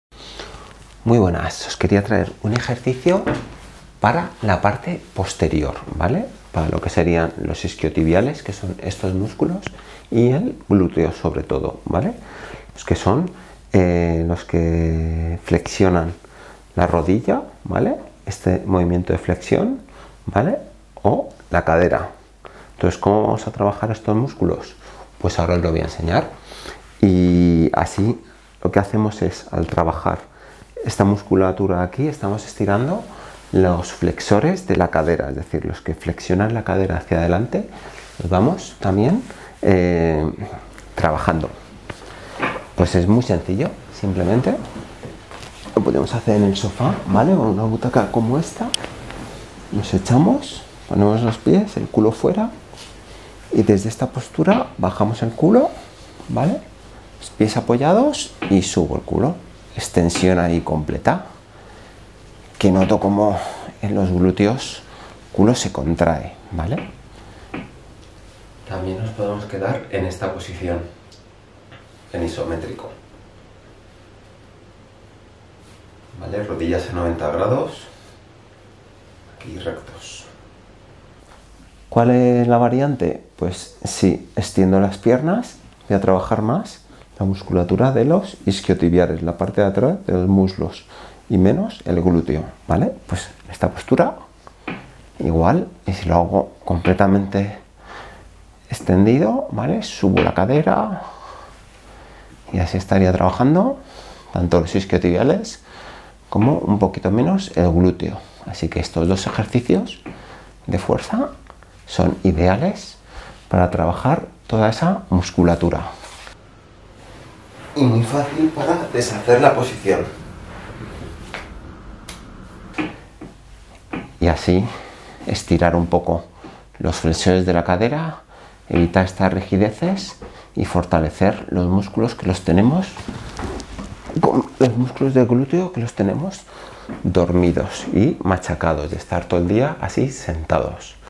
💪Ejercicios para GLÚTEOS EN CASA ❤️Rutina FUERZA entrenamiento funcional mujeres y mayores. Isquios
Ejercicio puente de glúteos e isquiotibiales en casa.